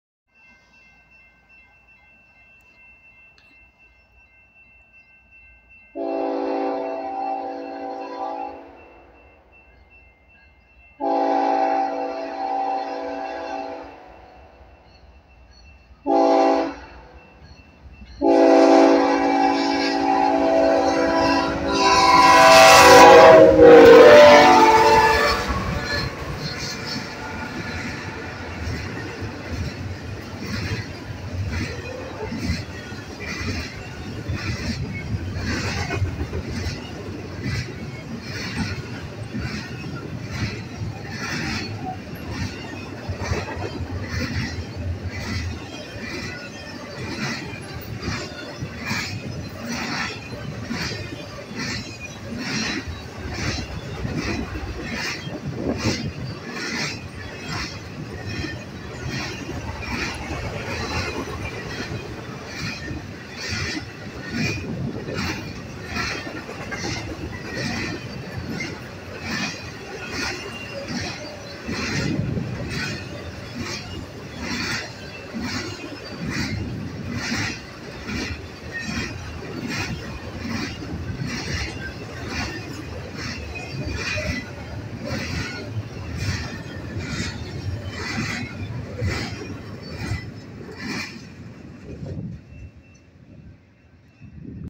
BNSF 7908 with a cool horn blast
EB Hotshot
BNSF 7908 H3 ES44DC Horn K5HL
BNSF 8252 H3 ES44C4
BNSF 5918 H3 ES44AC